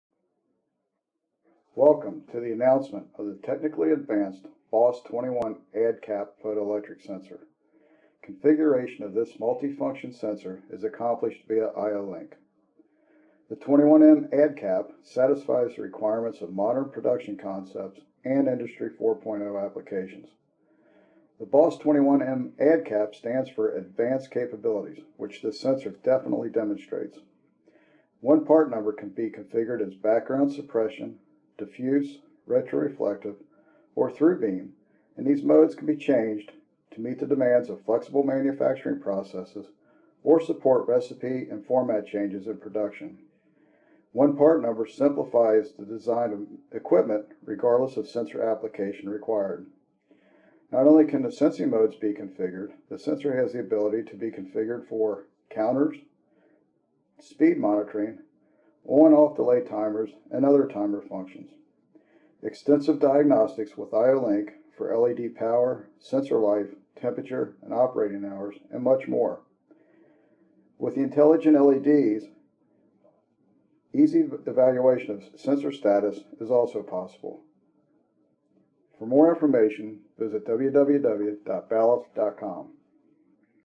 BOS 21M ADCAP
Introducing the new BOS 21M photoelectric multi-functional sensor with advanced capabilities.  One part number for all sensing modes.